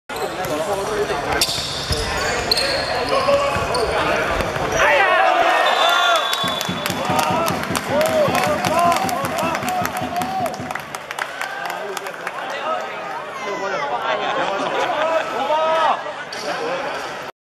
元朗vs深水埗(2013港運會五人足球決賽)片段12之入球2:1
11號梅開二度